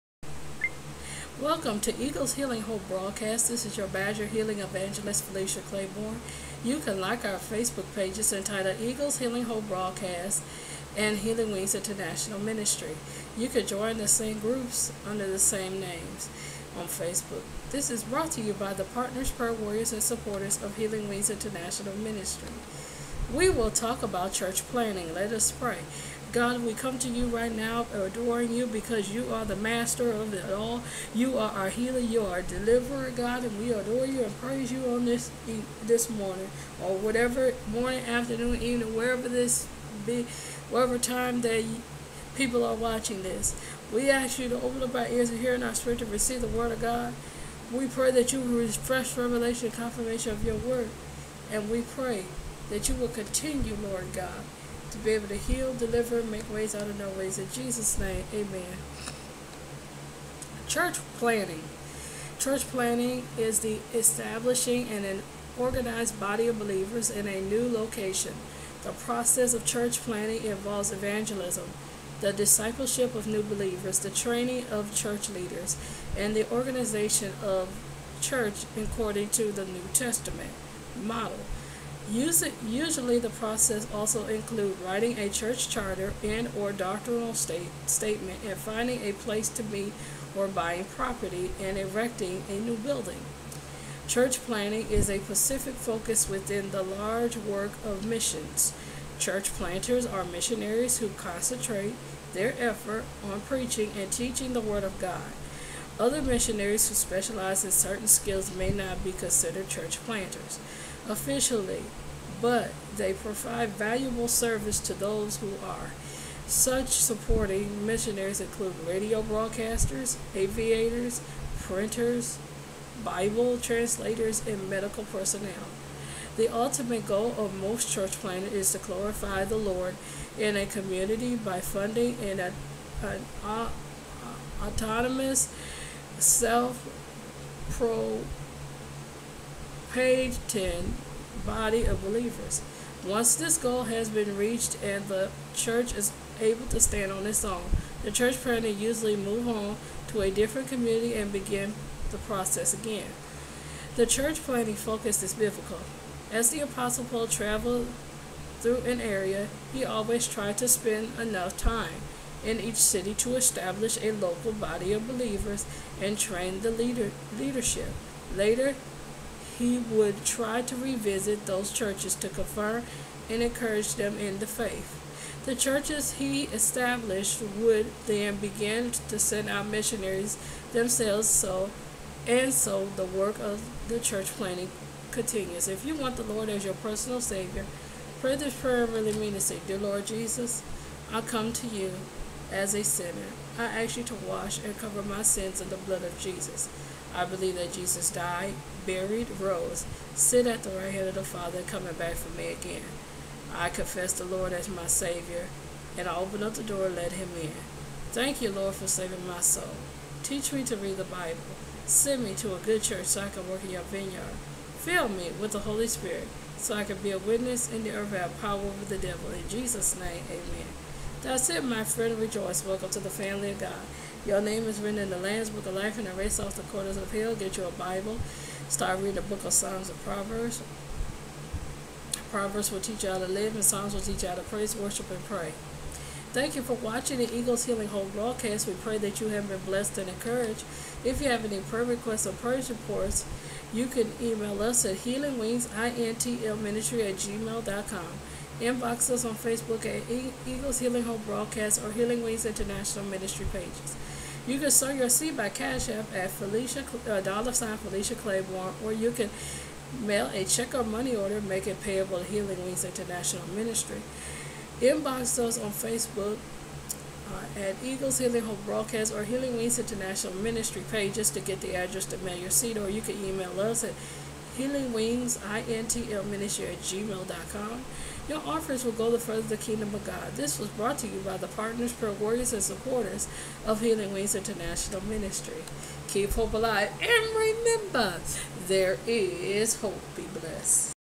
Church Planting
This is our eighty-third episode of Eagle's Healing Hope Broadcast talking about Church Planting.